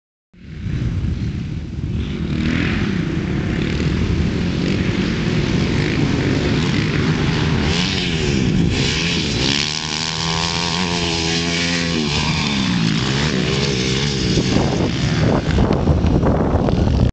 Hookin Up
Video taken on March 11, 2006 at Full Throttle MX in Athens, LA.  Beautiful Track and Setting in North Louisiana!